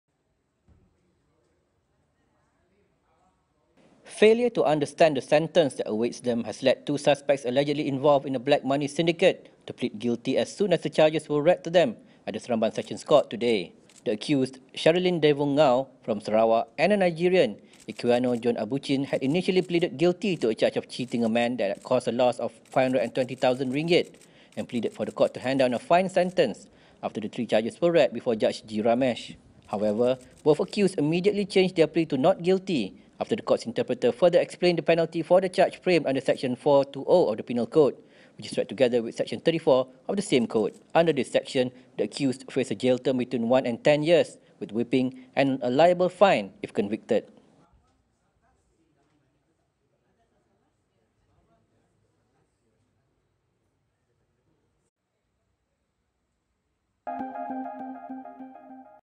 Duo, including Nigerian, charged over black money syndicate
Failure to understand the sentence that awaits them has led two suspects allegedly involved in a black money syndicate to plead guilty as soon as the charges were read to them at the Seremban Sessions Court today.